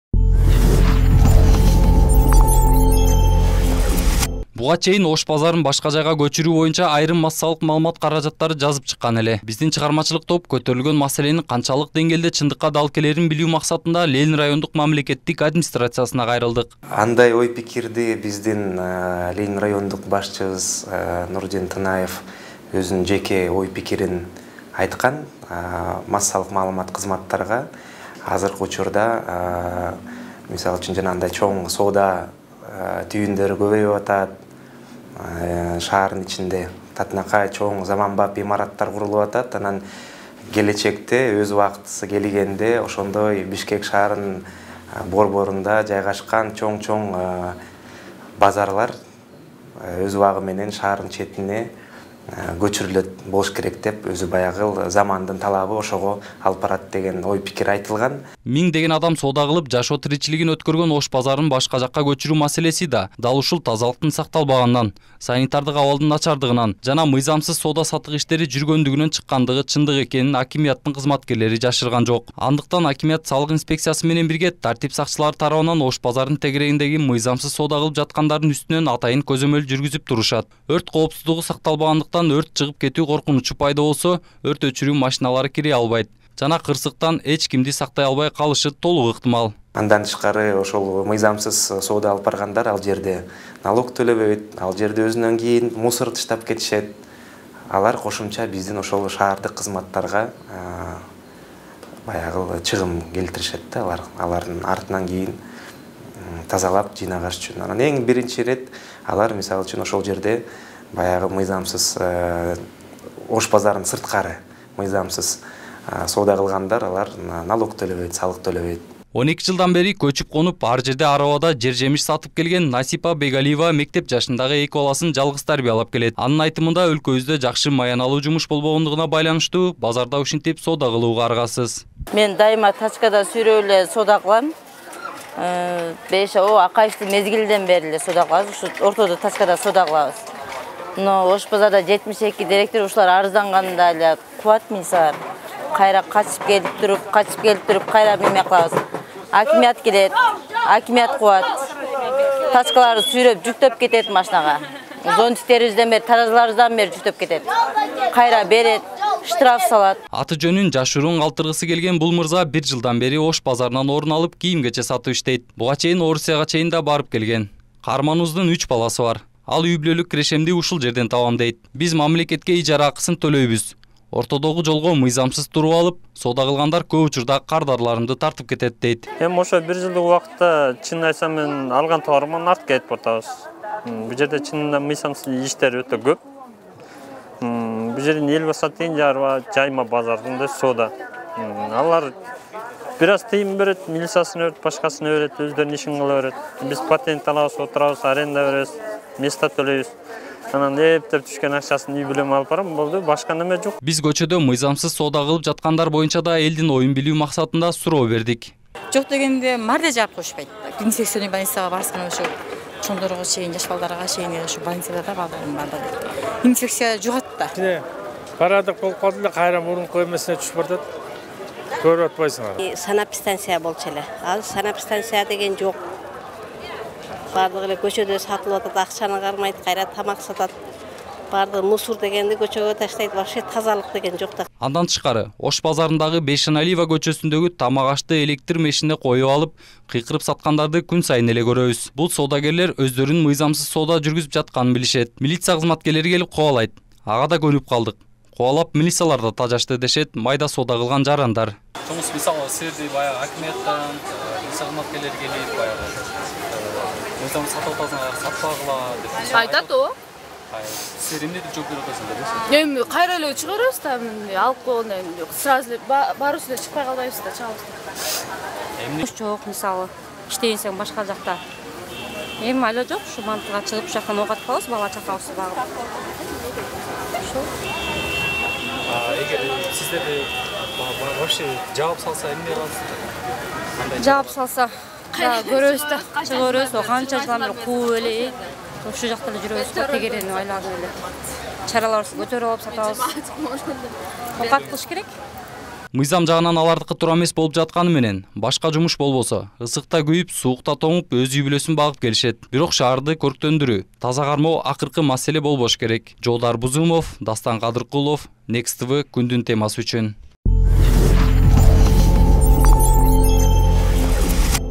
Бишкектеги Ош базары башка жайга көчүрүлөбү?
Буга чейин Ош базарын башка жайга көчүрүү боюнча айрым массалык маалымат каражаттары жазып чыккан эле. Биздин чыгармачылык топ көтөрүлгөн маселенин канчалык денгээлде чындыкка дал келээрин билүү максатында, Ленин райондук мамлекеттик администрациясына кайрылды.